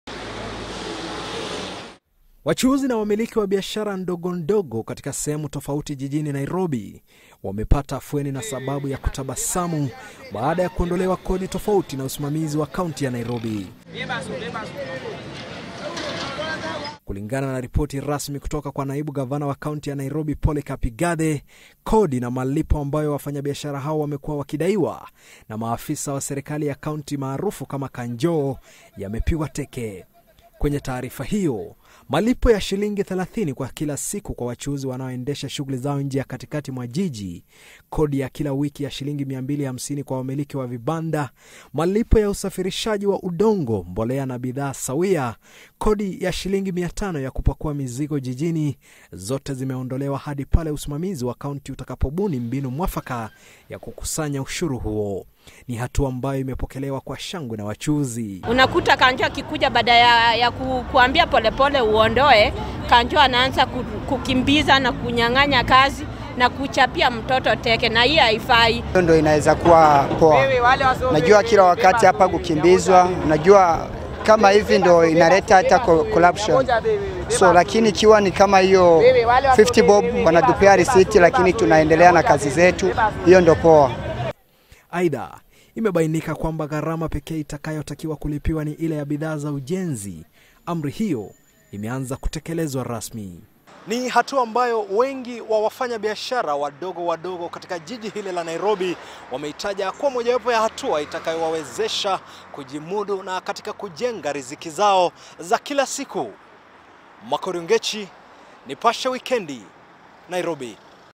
Kaunti ya Nairobi yaondoa baadhi ya ushuru
Serikali ya kaunti ya Nairobi imefutilia mbali malipo na kodi tofauti kwa wafanyabiashara wadogo wadogo katika kaunti ya Nairobi. Kwa mujibu wa barua iliyoandikwa na naibu gavana wa Nairobi Polycarp Igathe, amri hiyo ilipaswa kutekelezwa kuanzia tarehe 14 mwezi uliopita.